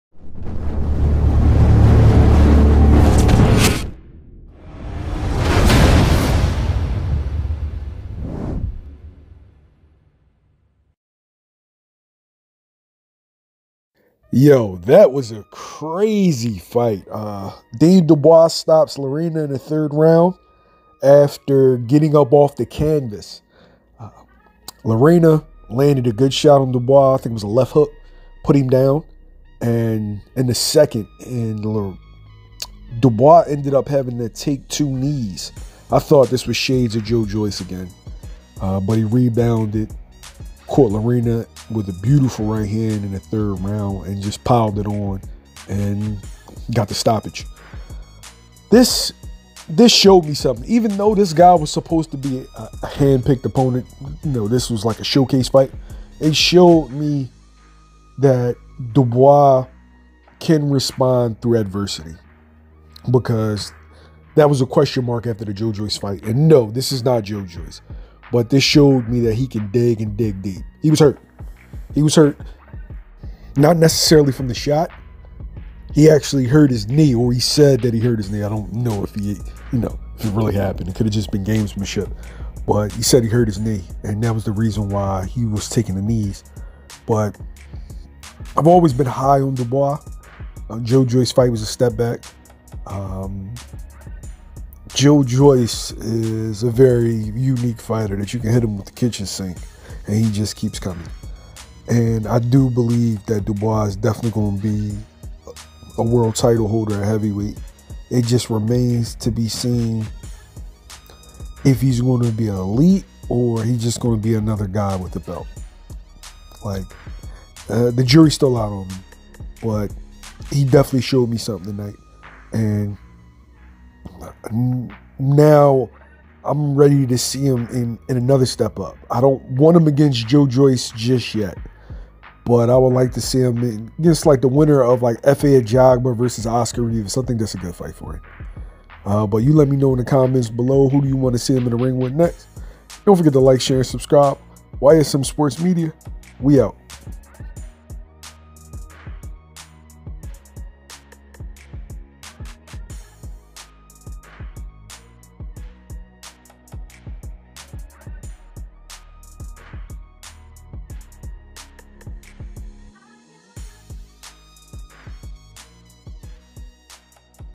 (🤯🤯🤯) Daniel Dubois gets off the canvas 3 tines before stopping Kevin Lerena in the 3rd round!!!!
Heavyweight Contender Daniel Dubois gets knocked down 3 times in the 2nd round to defeat Kevin Lerena by 3rd round TKO.